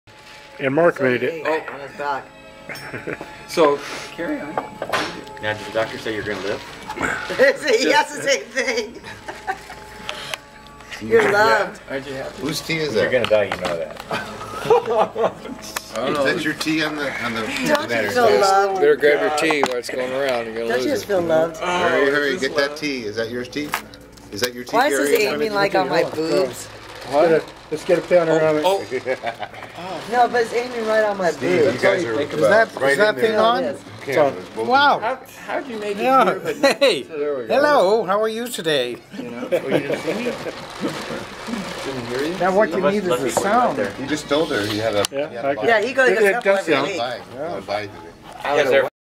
Mench lunch2.AVI
Murry took us out for lunch.